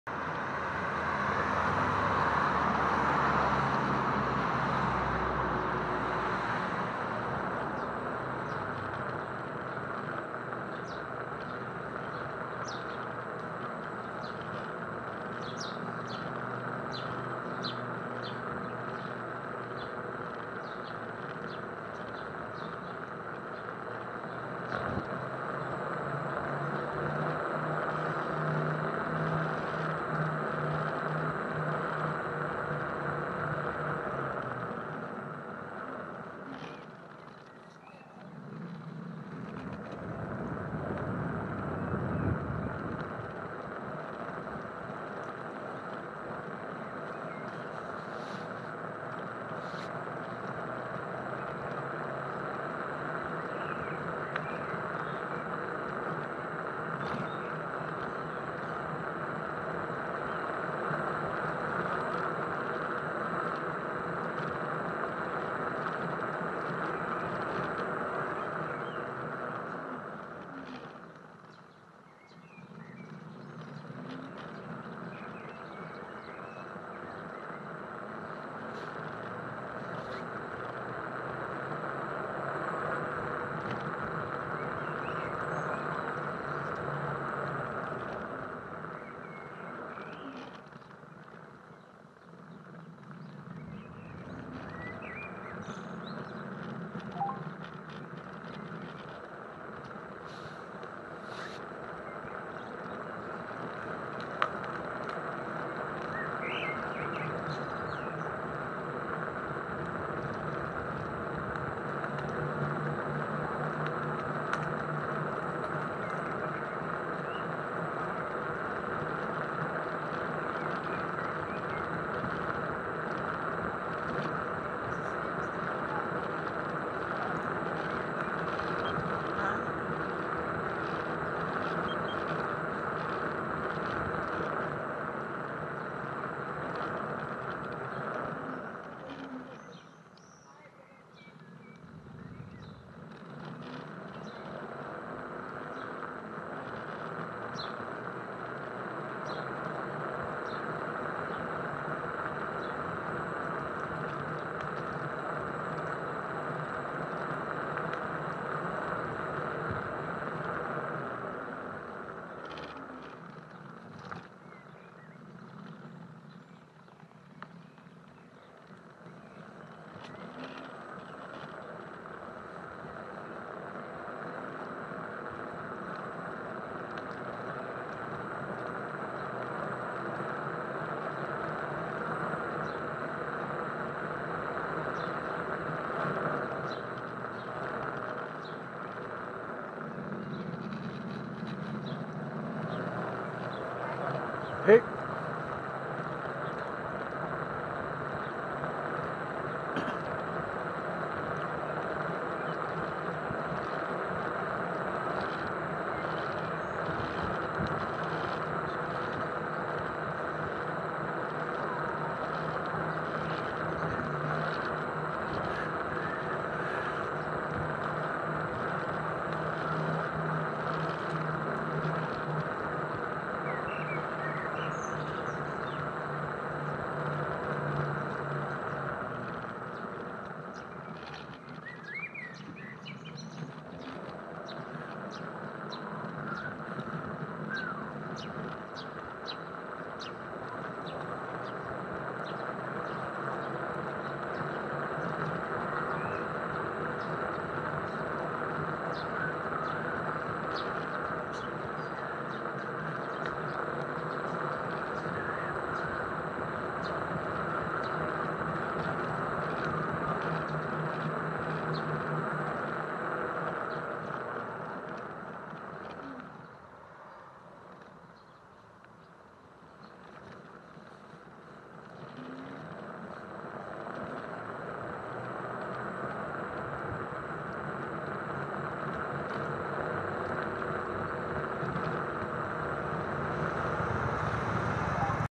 Let´s take a ride!
Join me for a ride in the quiet suburbs of Copenhagen.
This version is 3D 180 VR anaglyph.